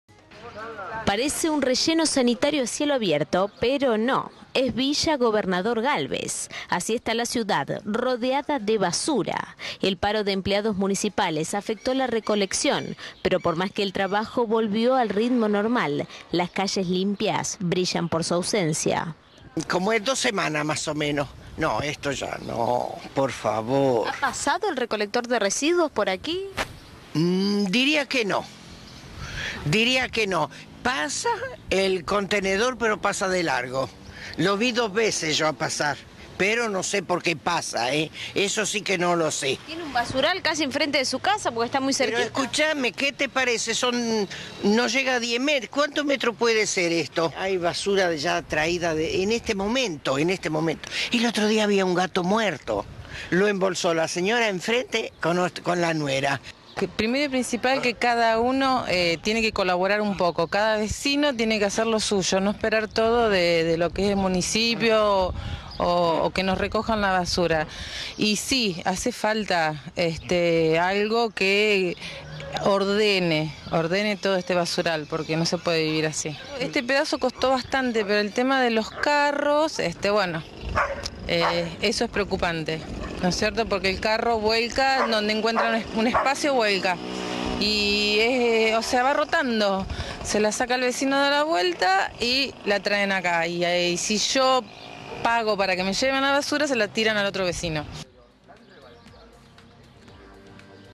SOCIEDAD . Continúan los problemas de recolección en Gálvez
Pese al levantamiento del paro de municipales, la basura continúa en las calles y los vecinos se quejan por la suciedad y la falta de higiene en la ciudad.